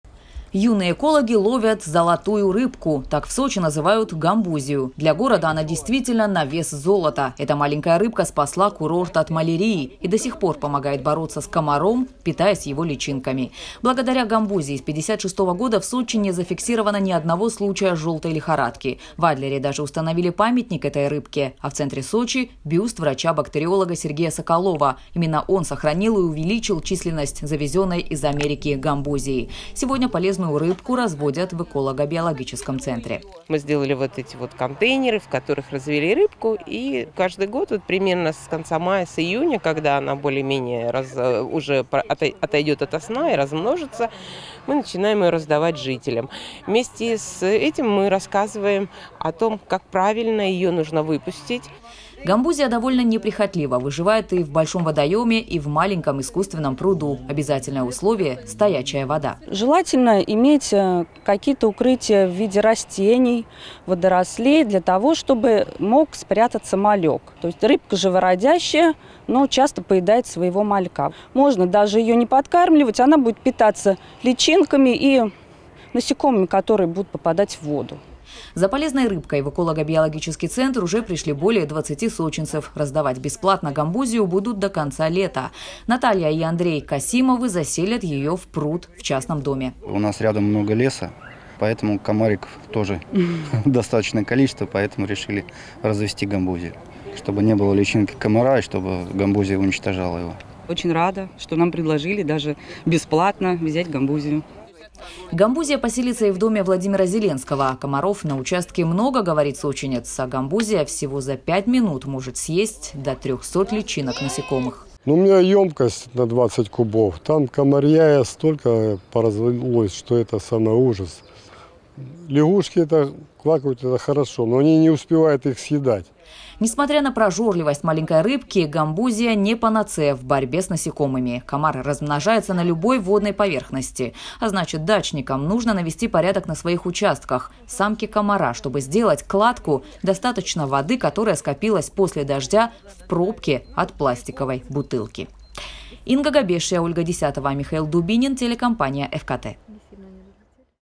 В Сочи бесплатно раздают гамбузию. Новости Эфкате
Эта маленькая рыбка питается личинками комаров, а значит помогает бороться с этими назойливыми насекомыми. Получить гамбузию можно в Эколого-биологическом центре на ул. Альпийской.

Гамбузию называют «золотой рыбкой» Сочи. Эта маленькая рыбка спасла курорт от малярии. И до сих помогает бороться с комаром, питаясь его личинками. Благодаря гамбузии с 56-го года в Сочи не зафиксировано ни одного случая желтой лихорадки. В Адлере даже установили памятник этой рыбке. А в центре Сочи — бюст врача-бактериолога Сергея Соколова — именно он сохранил и увеличил численность завезенной из Америки гамбузии. Сегодня полезную рыбку разводят в Эколого-биологическом центре.